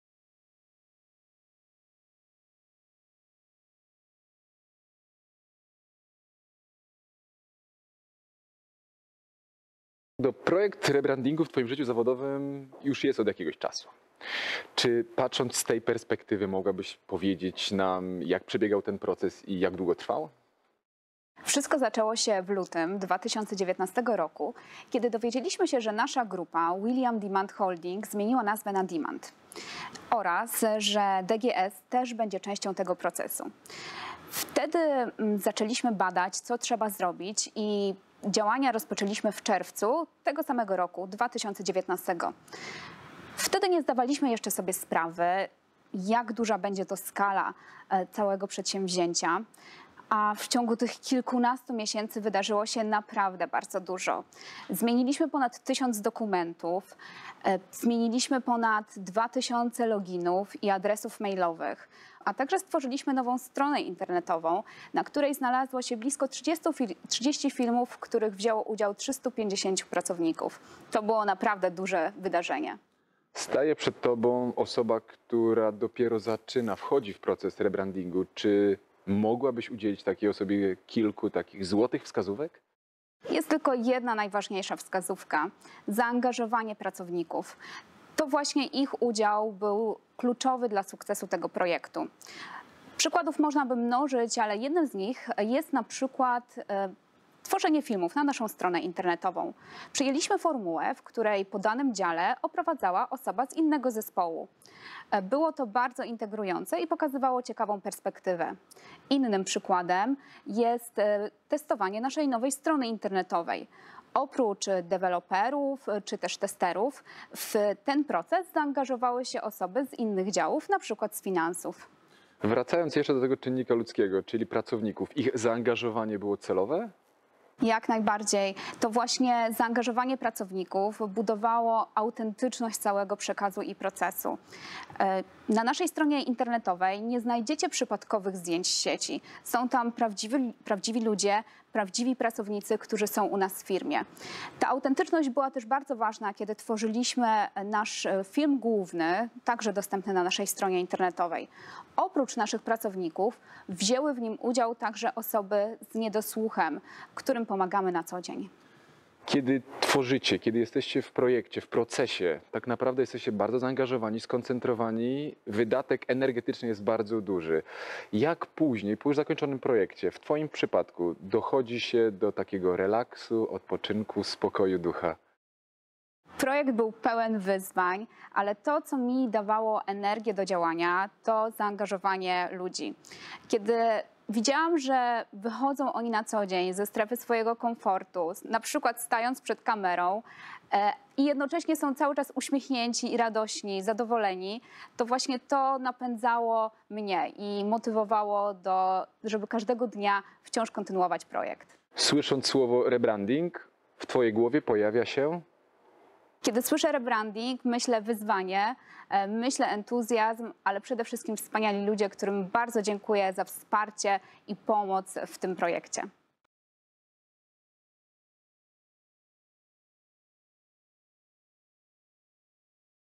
Demant TV - wywiad z Magdą
Projekt „Rebranding” to ogromne przedsięwzięcie. Zmiana ponad 1000 dokumentów, stworzenie 2000 nowych adresów mailowych, nagranie ponad 30 nowych filmów, czy stworzenie nowej strony www to dopiero wierzchołek góry lodowej całego projektu. Podstawą i paliwem napędowym, jest zaangażowanie pracowników, o którym opowiada Magdalena, lider projektu „Rebranding”.